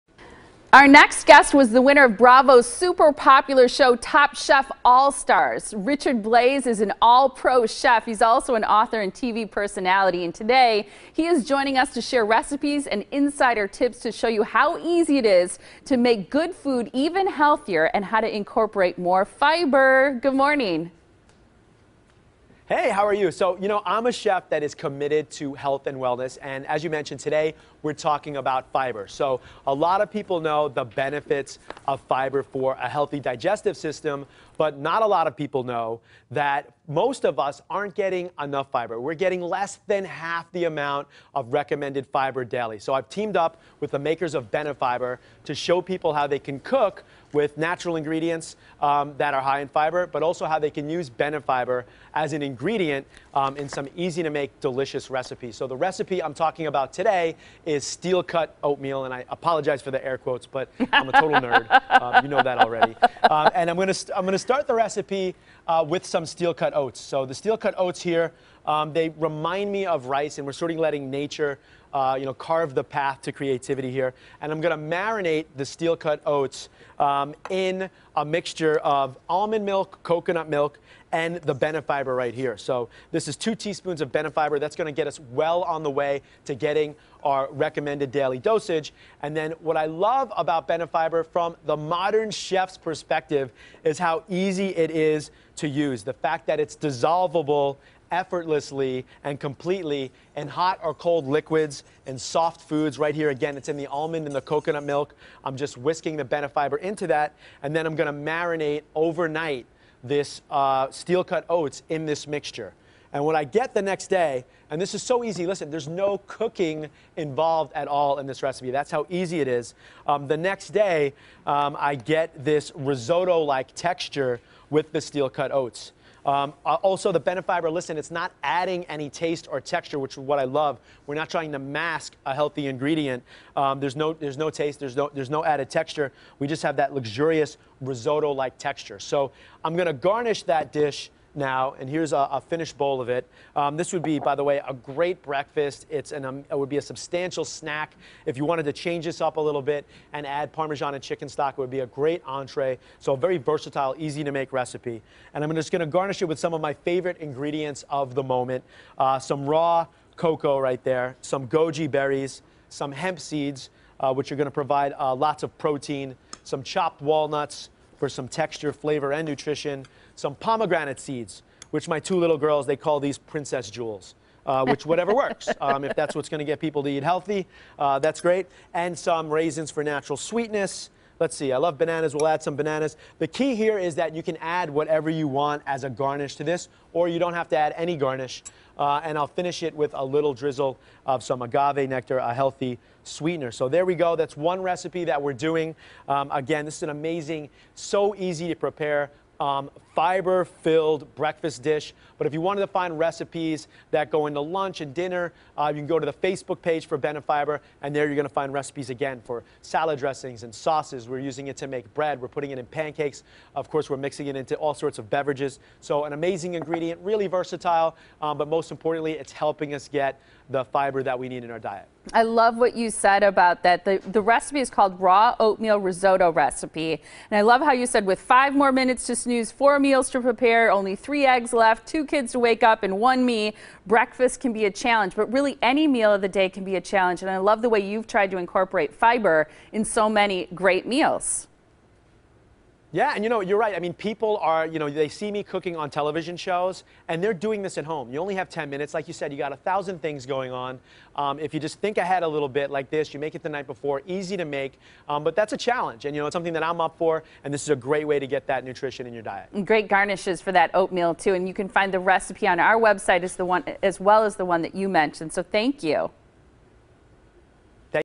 Help Make Favorite Recipes Healthier
According to the Academy of Nutrition and Dietetics, most American's aren't consuming enough fiber.  Top Chef All Stars Winner Richard Blais joins us to share his favorite healthy recipes and some cooking tips and tricks to get more fiber in your diet without sacrificing taste.

 Richard Blais Raw Oatmeal Risotto Recipe

      With 5 more minutes to snooze, 4 meals to prepare, only 3 eggs left, 2 kids to wake up and 1 me, breakfast can definitely be a challenge. With so many grab and go meals available